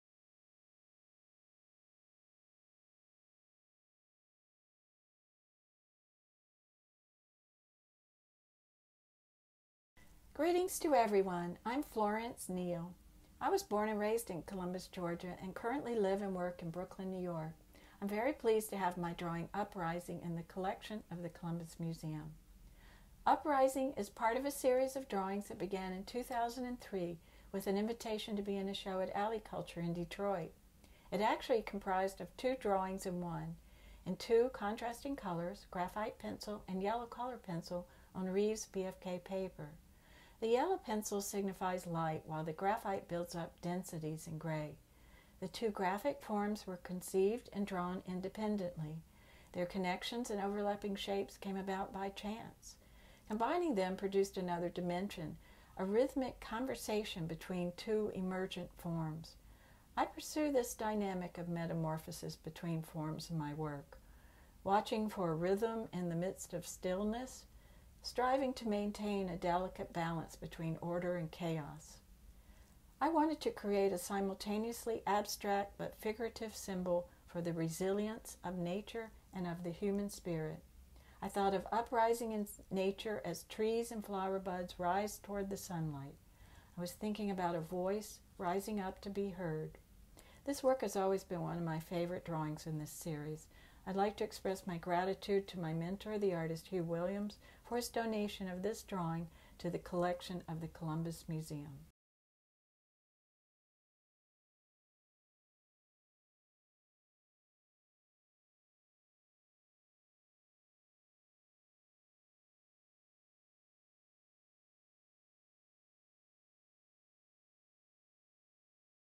60 Seconds with Florence Neal
Take 60Seconds with Florence Neal as the artist discusses how her drawing "Uprising," 2003 in the permanent collection of The Columbus Museum is about rhythm, stillness, and a "balance between order and chaos."